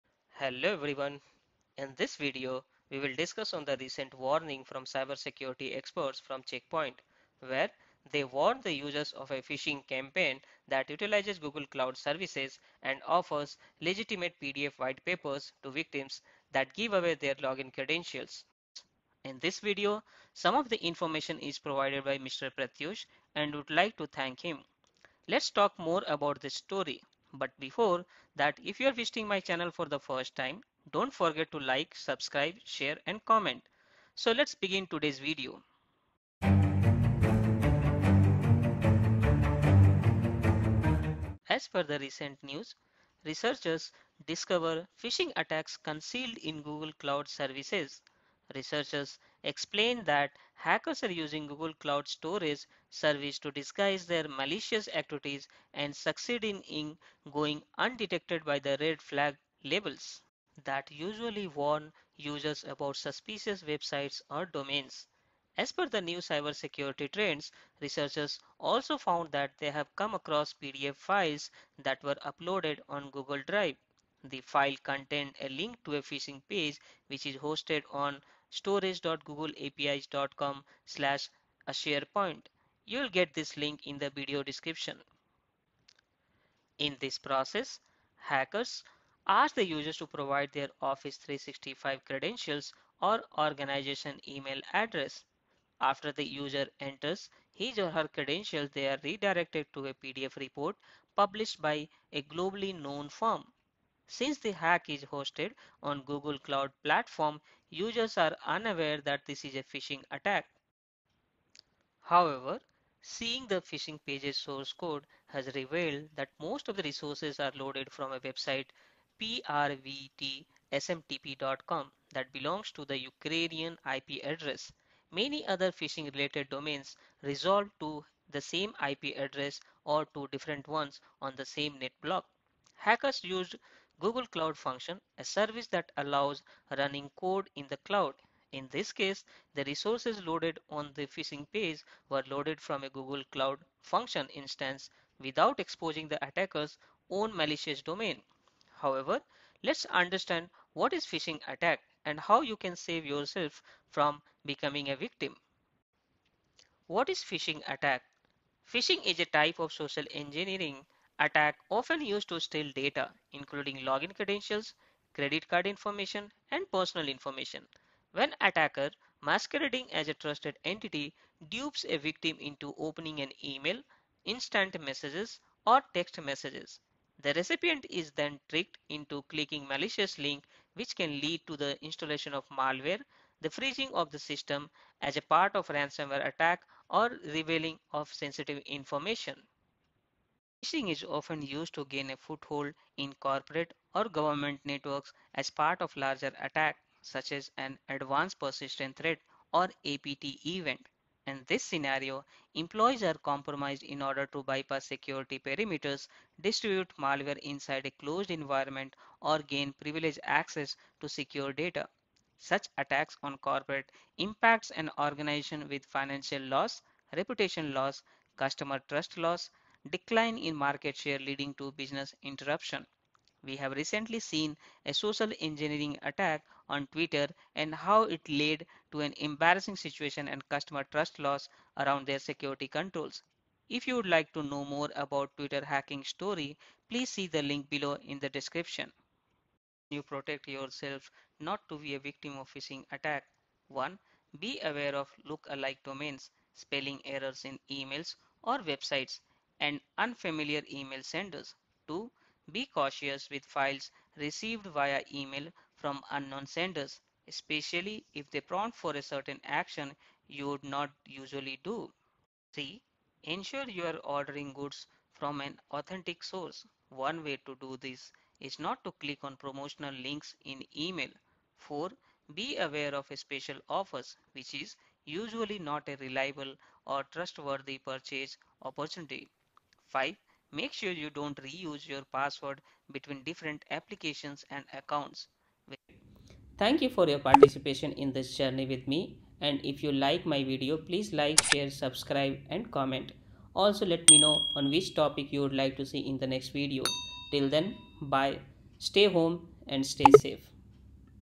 How Google Cloud Service Platform Hacked🔥🔥🔥
This video is about the recent Phishing attack on Google Cloud Service platform where the login credentials of users were stolen.

The recent warning from cyber security researchers in Check Point, where they warn the users of a phishing campaign that utilizes Google Cloud services and offers legitimate PDF white papers to victims that give away their login credentials.
In this video some of the information is provided by Mr Prathyush and would like to thank him.
As per the recent news, researchers discover phishing attacks concealed in google cloud services.

Researchers explained that hackers are using google cloud storage service to disguise their malicious activities and succeeding in going undetected by the red flag labels, that usually warn users about suspicious websites or domains.

As per the new cyber security trends, researchers also found that they have come across PDF files that were uploaded on
google drive. The file contained a link to a phishing page which is hosted

After the user enters His/her credentials they are redirected to a pdf report published by a globally known firm. Since the hack is hosted on Google cloud platform, users are unaware that this is phishing attack.

However, seeing Recording  the phishing pages source code has revealed that most of the resources are loaded from a website prvtsmtp.com that belongs to the Ukrainian IP address. (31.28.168.XXX), many other phishing related domains resolved to the same IP address or to different ones on the same Netblock.

Hackers used google cloud function, a service that allows running code in the cloud. In this case, the resources loaded on the phishing page were loaded from a google cloud function instance without exposing the attackers own malicious domain.
However, let's understand what is phishing attack and how you can save yourself from becoming a victim.
Phishing attack –

Phishing is a type of social engineering attack often used to steal data, including login credentials, credit card information, Personal information.
When attacker masquerading as a trusted entity, dupes a victim into opening an email, instant messages or text messages. The recipient is then tricked into clicking malicious link, which can lead to the installation of malware, the freezing of the systems as part of ransomware attack or the revealing of sensitive information.Phishing is often used to gain a foothold in corporate or government networks as part of larger attack, such as an Advanced Persistant threat (APT) event.

In this scenario employees are compromised in order to bypass security perimeters, Distribute malware inside a closed environment or gain privileged access to secure data.

Such attacks on corporate impacts an organization with financial loss, reputation loss, customer trust loss, decline in market share leading to business interruption.

We have recently had a Social engineering attack on twitter and how it led to an embarrassing situation and customer trust loss around their security controls.

If you would like to know more twitter hacking story please see the link below in the description.

How can you protect yourself not to be a victim of Phishing attack
Be aware of lookalike domains, spelling errors in emails or websites, and unfamiliar email senders
Be cautious with files received via email from unknown senders, especially if they prompt for a certain action you would not usually do
Ensure you are ordering goods from an authentic source. One way to do this is NOT to click on promotional links in email
Be aware of special offers, which is usually not a reliable or trustworthy purchase opportunity.
Make sure you don’t reuse your passwords between different applications and accounts